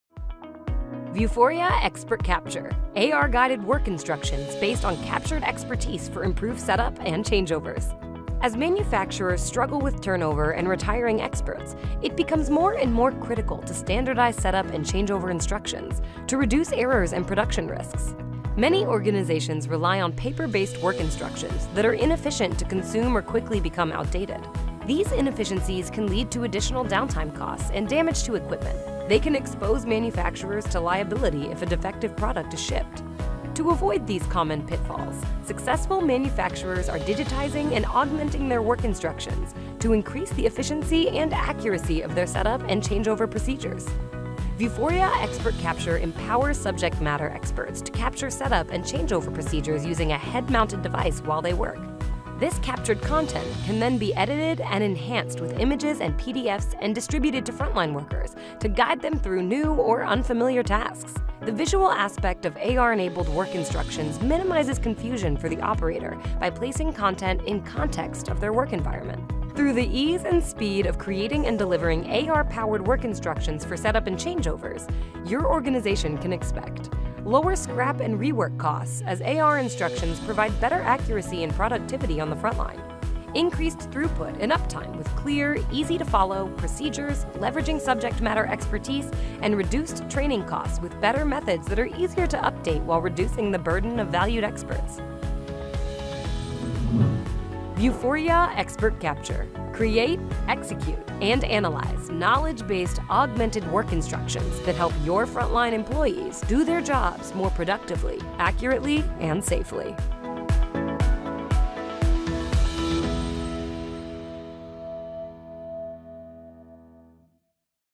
Improve machine set-up and changeover procedures with Vuforia Expert Capture
Lack of standardized instructions/SOPs can cause errors or inefficiencies during machine and line setup and changeover. These inefficiencies can lead to additional downtime costs and damage to equipment. AR-enabled work instructions minimize confusion for the operator by placing content in context of their work environment. AR instructions provide better accuracy and productivity on the frontline reducing waste, improving changeover efficiency and increasing uptime.